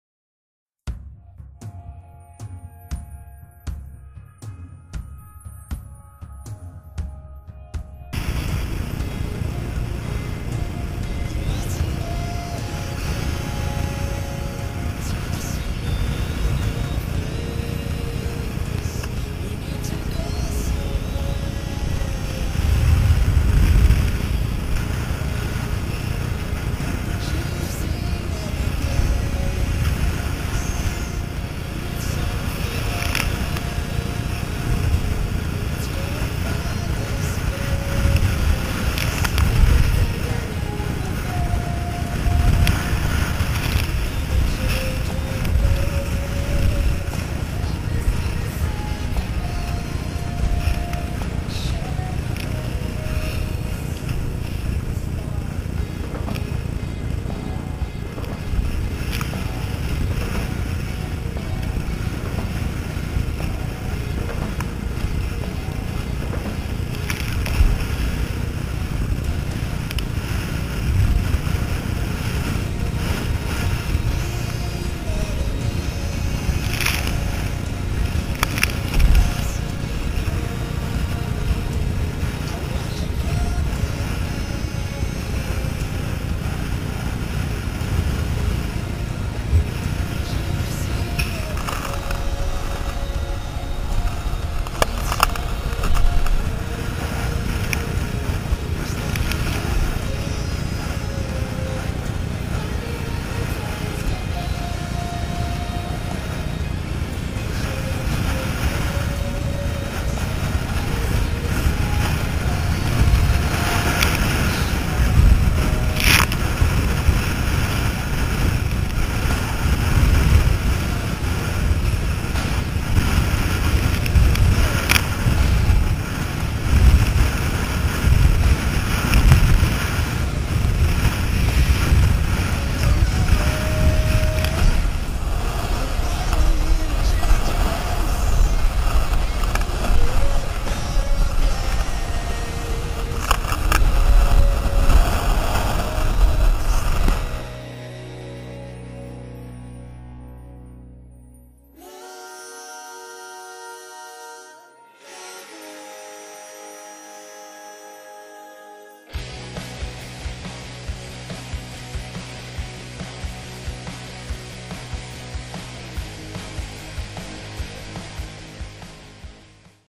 Windy day
Ocean paddling Vesteraalen, Norway, may 25, 2009 - gail